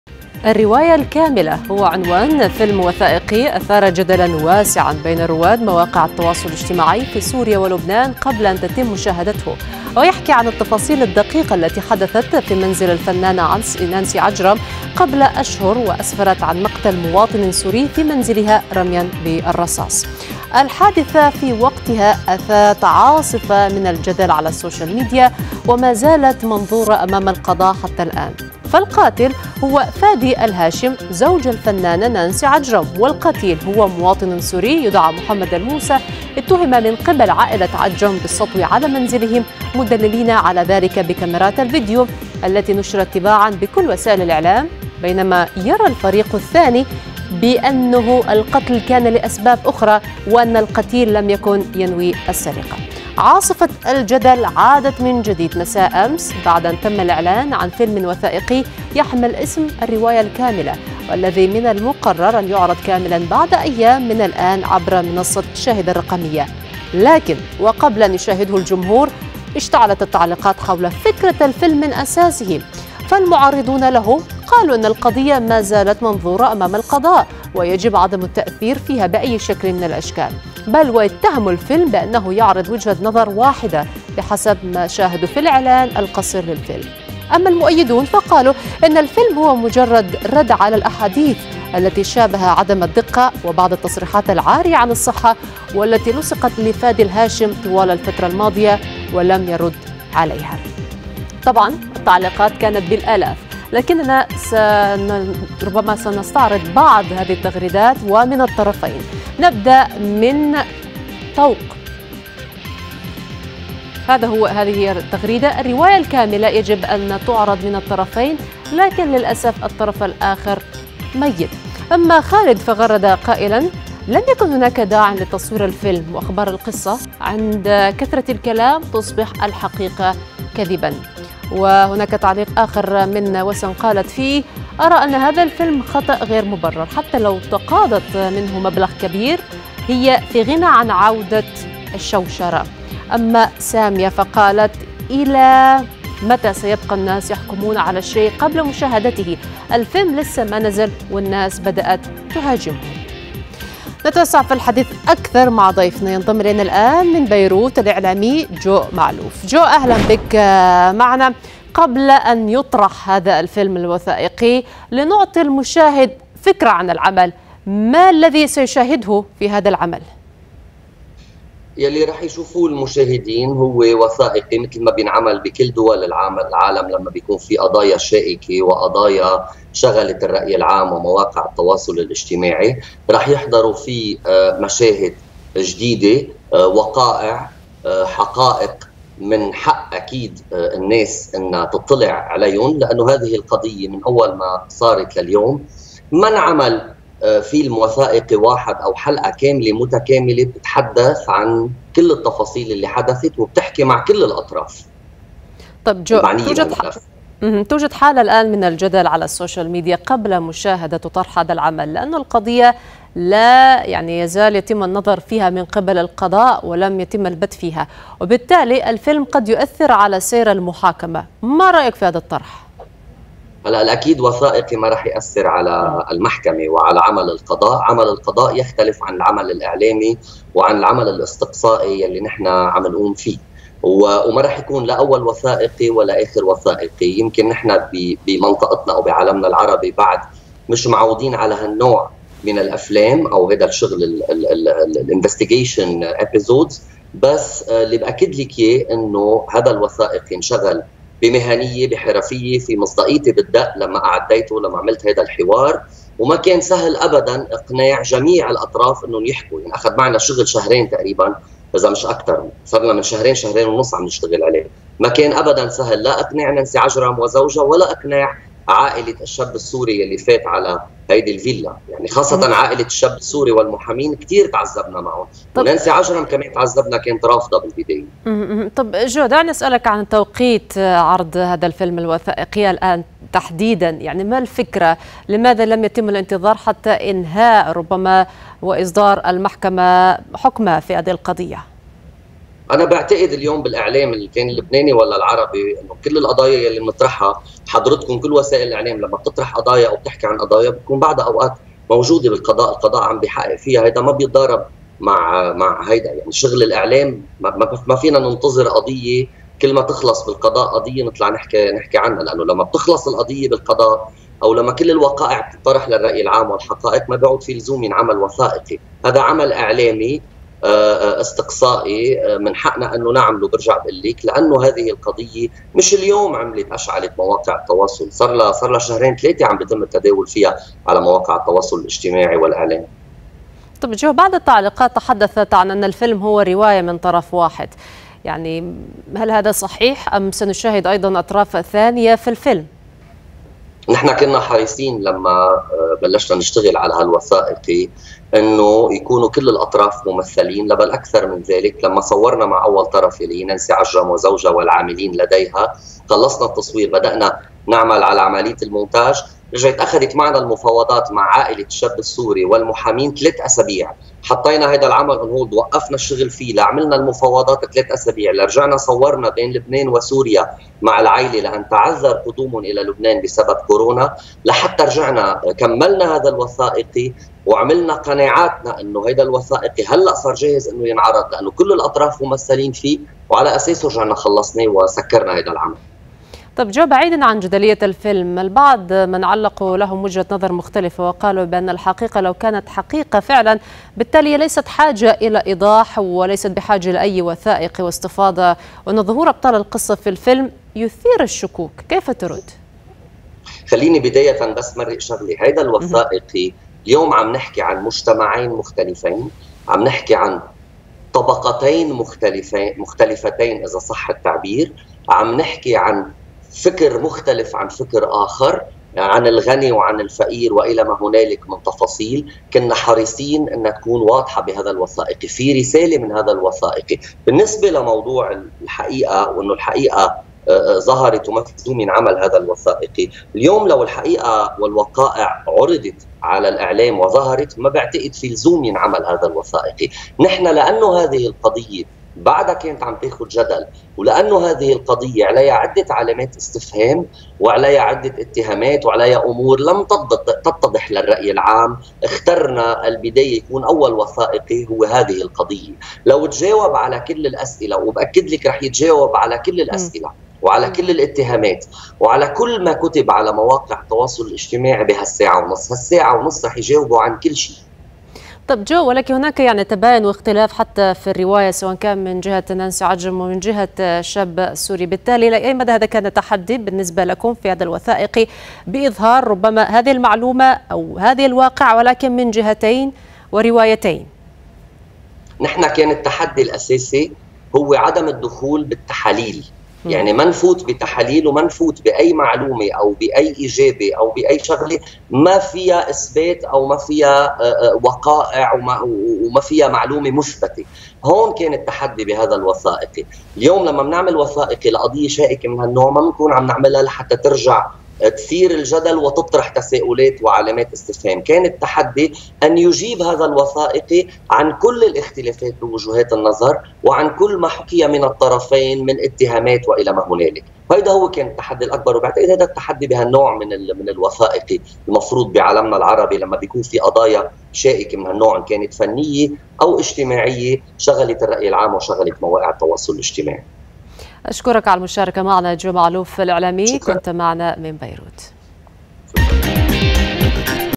جدل حول فيلم الرواية الكاملة لجريمة منزل نانسي عجرم | منصات
جدل حول فيلم الرواية الكاملة لجريمة منزل نانسي عجرم